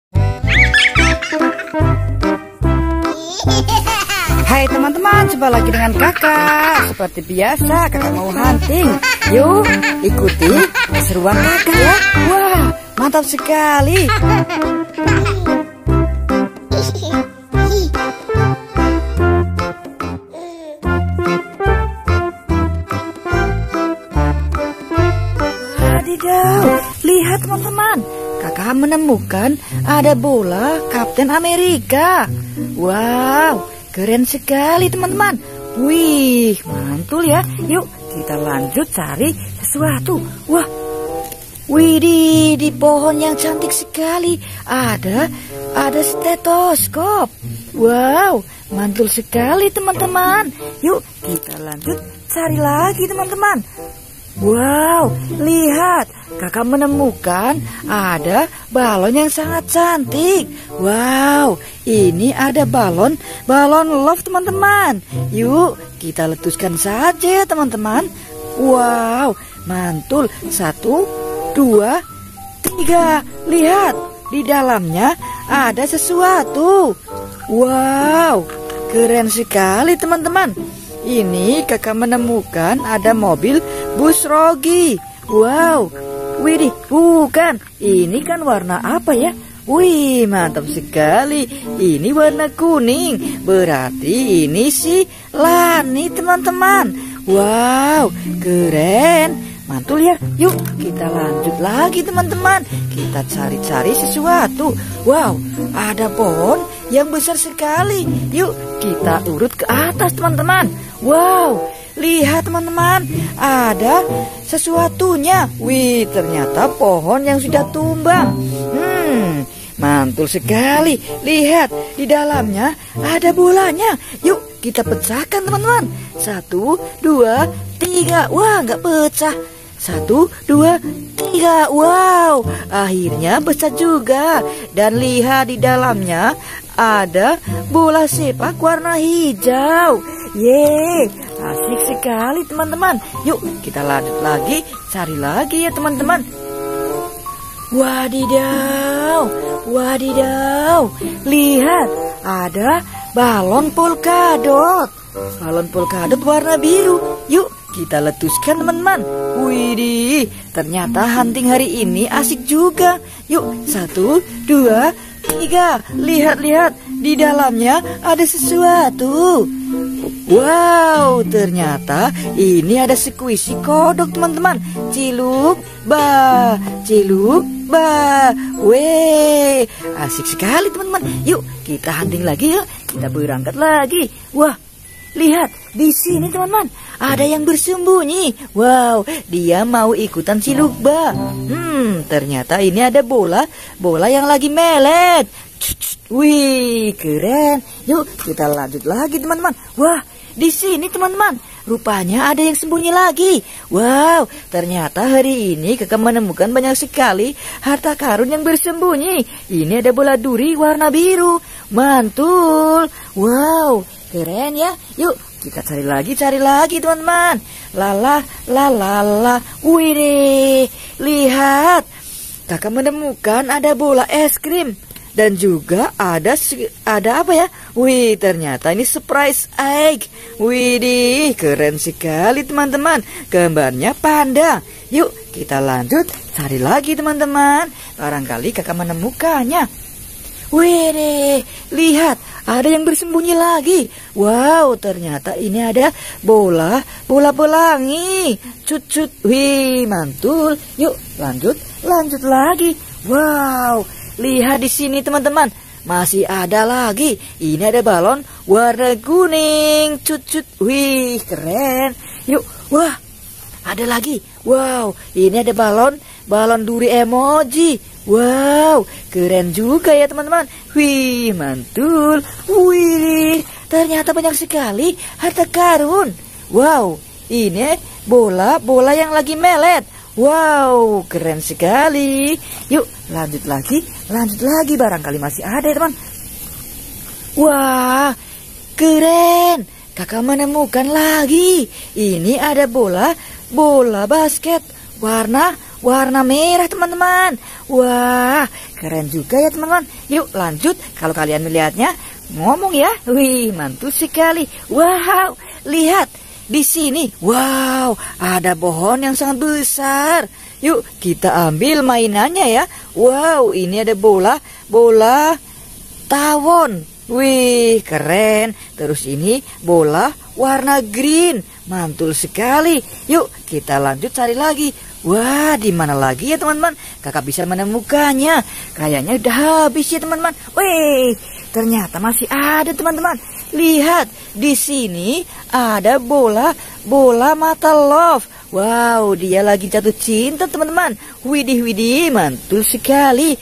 ASYIIIIK MENEMUKAN BANYAK BOLA DAN BALON, BALON WARNA WARNI, BALON KARAKTER, BOLA EMOJI, BOLA BASKET
yuk teman teman ikuti keseruan disini.